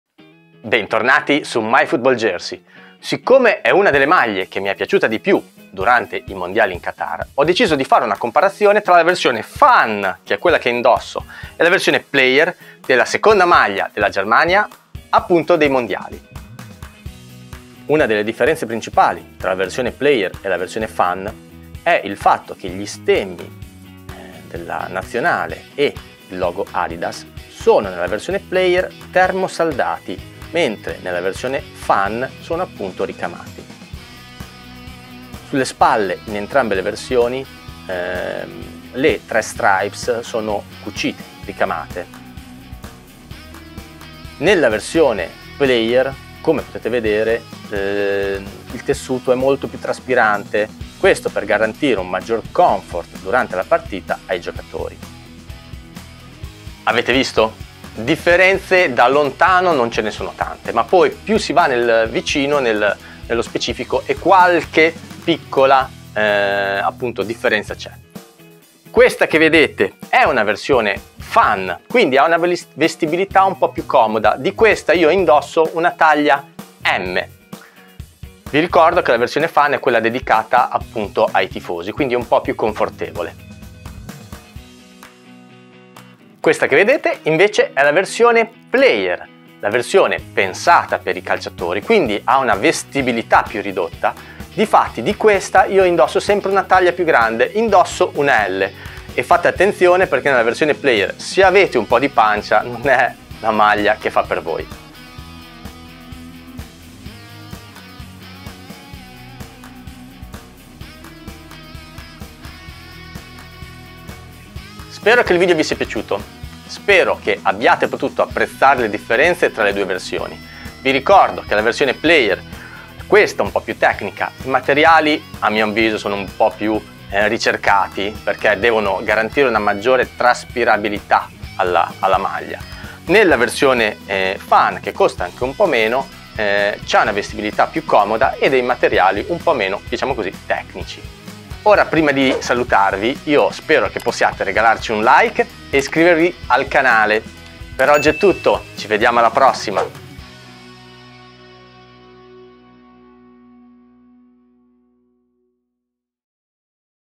Germany Away Jersey Qatar 2022 – Player Version Vs Fan Version
Discover the differences between the Player Version and the Fan Version of the Qatar 2022 Germany Away Jersey
Watch till the end to see how the Germany Away Jersey Qatar 2022 fits!

🕘 Timecodes
00:00 Overview
00:17 Details View
00:56 Conclusion

Please watch the Germany Away Jersey Qatar 2022 – Player Version Vs Fan Version video review in 1080p (HD) 📺

Follow me also on TikTok @my.football.jersey to watch the Germany Away Jersey Qatar 2022 – Player Version Vs Fan Version video review in a different way. 💥